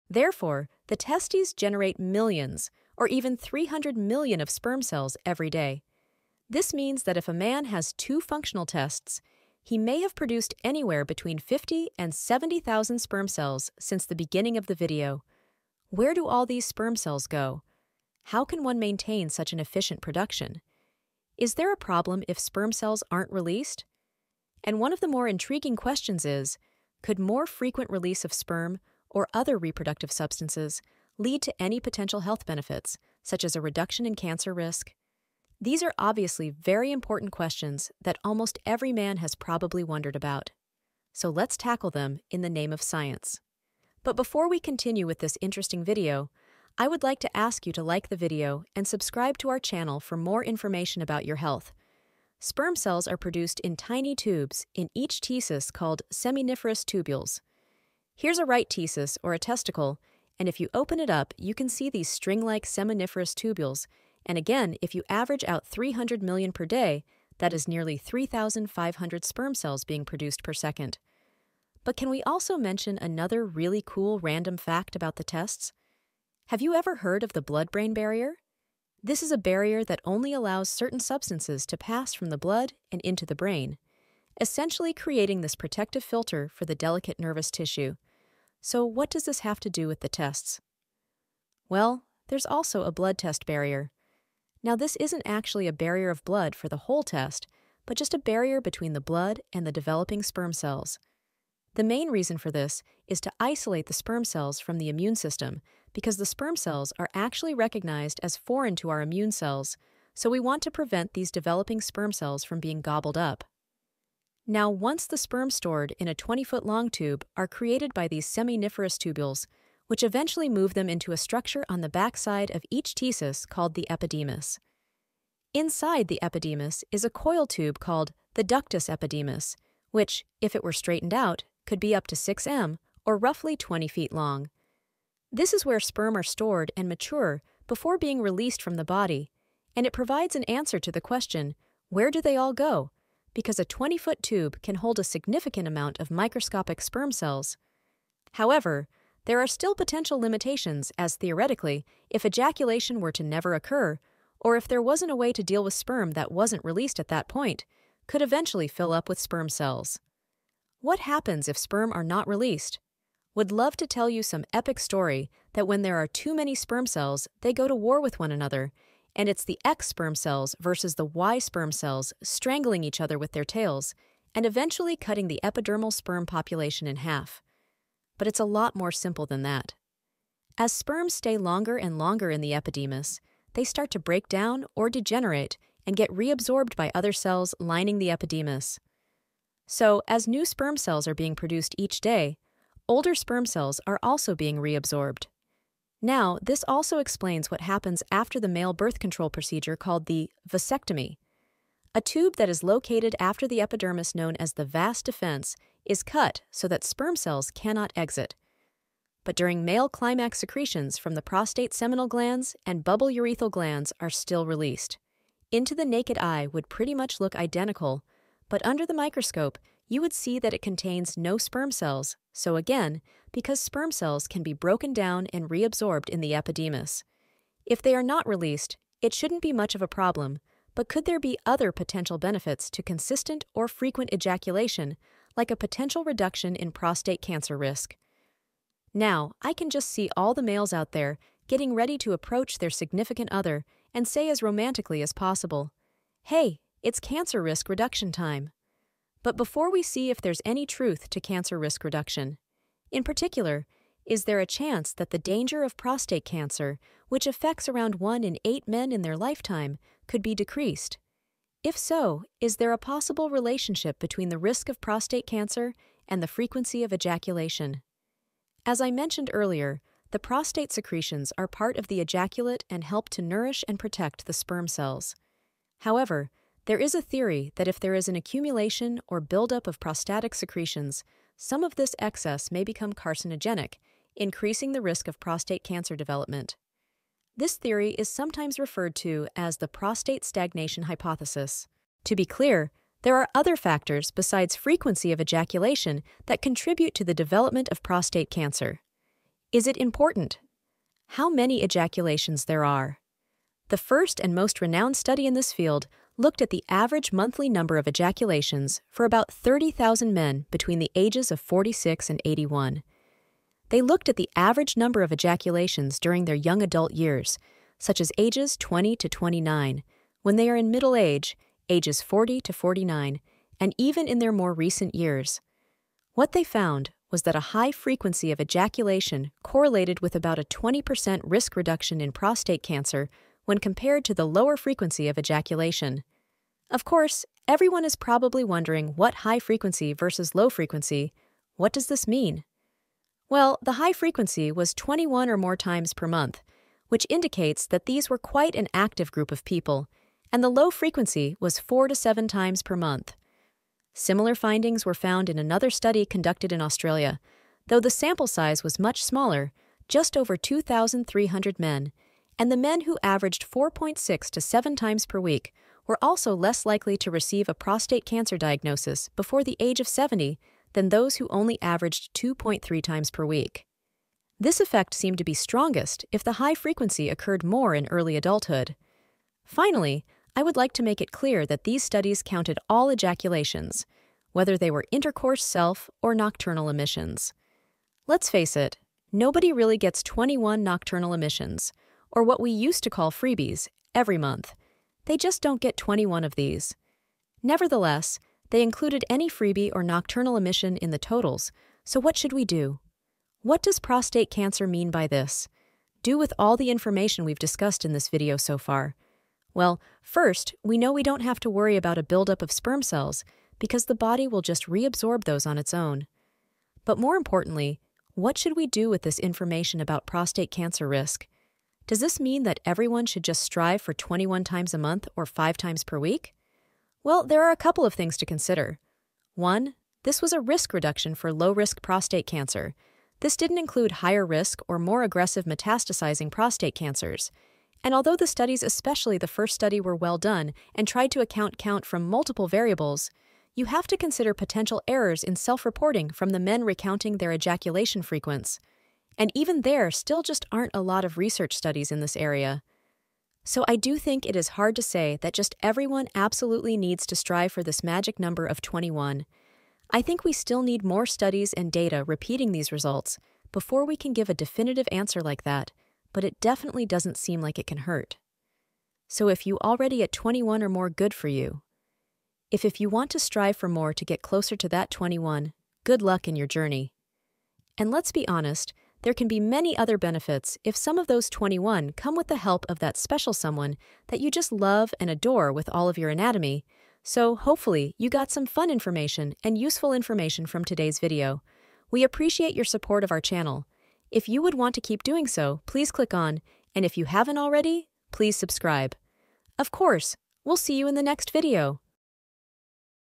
Does Masturbation Reduce Prostate Cancer? You Want To Know THESE facts!
human body, prostate cancer

Therefore, the testes generate millions, or even 300 million of sperm cells every day.

This means that if a man has two functional testes, he may have produced anywhere between 50 and 70,000 sperm cells since the beginning of the video.
Where do all these sperm cells go?
How can one maintain such an efficient production?
Is there a problem if sperm cells aren't released?
And one of the more intriguing questions is, could more frequent release of sperm or other reproductive substances lead to any potential health benefits, such as a reduction in cancer risk?
These are obviously very important questions that almost every man has probably wondered about. so let's tackle them in the name of science

sperm cells are produced in tiny tubes in each tesus called seminiferous tubules. here's a right tesus or a testicle and if you open it up you can see these string-like seminiferous tubules and again if you average out 300 million per day that is nearly 3500 sperm cells being produced per second. but can we also mention another really cool random fact about the testes have you ever heard of the blood brain barrier? this is a barrier that only allows certain substances to pass from the blood and into the brain essentially creating this protective filter for the delicate nervous tissue, so what does this have to do with the testes? well there's also a blood test barrier. now this isn't actually a barrier of blood for the whole test but just a barrier between the blood and the developing sperm cells. the main reason for this is to isolate the sperm cells from the immunesystem , because the sperm cells are actually recognized as foreign to our immune cells so we want to prevent these developing sperm cells from being gobbled up. now once the sperm Stored In a 20 Foot Long Tube
are created by these seminiferous tubules, which eventually move them into a structure on the back side of each tesus called the epidemis. Inside the epidemis is a coil tube called the ductus epidemis, which, if it were straightened out, could be up to 6 M, or roughly 20 feet long.
This is where sperm are stored and mature before being released from the body and it provides an answer to the question, "Where do they all go?"
because a 20-foot tube can hold a significant amount of microscopic sperm cells.

About Human body institute
🎥 Videos about Health, human body
🎨 Written, voiced and produced by human body institute